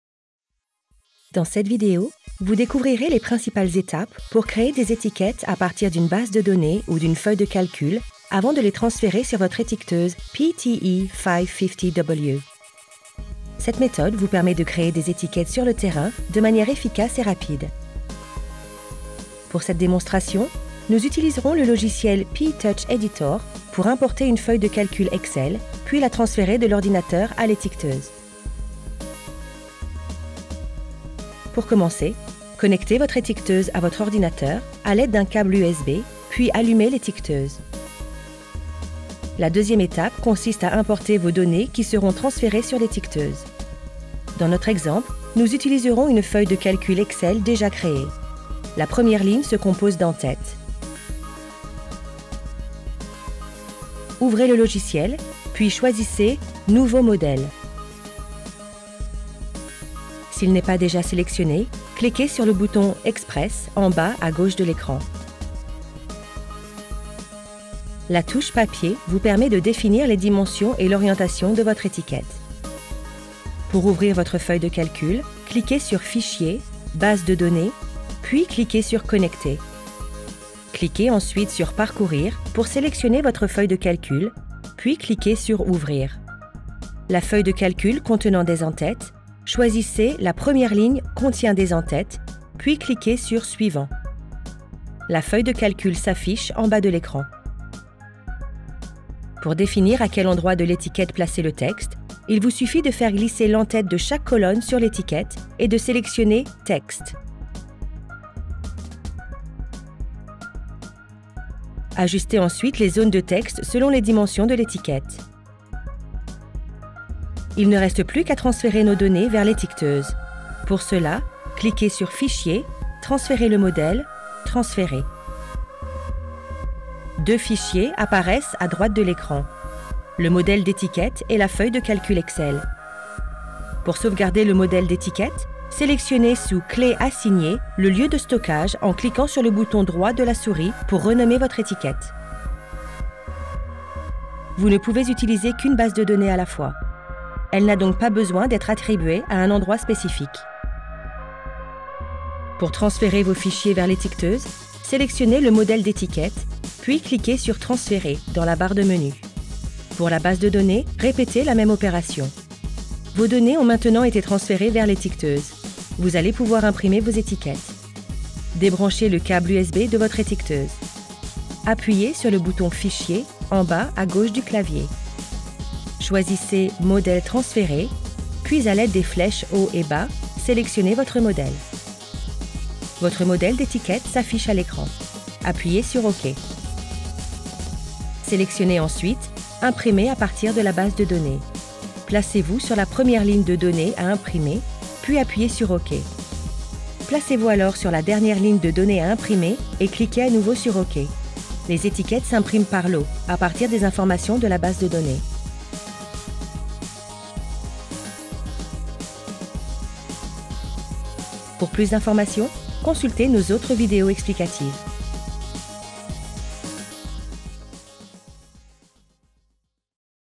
Créer des étiquettes en toute simplicité | Brother
Créez des étiquettes à partir d'une base de données ou d'une feuille de calcul, avec votre étiqueteuse PT-E550WVP.